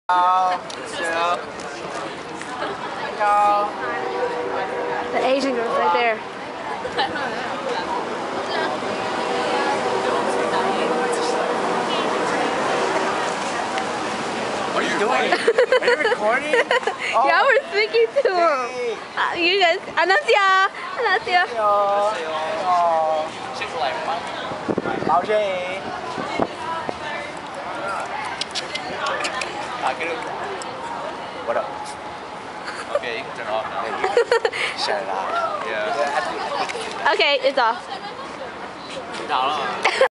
Stephen & Peter Tryna Hola At Korean Girls
@ the World of Coke!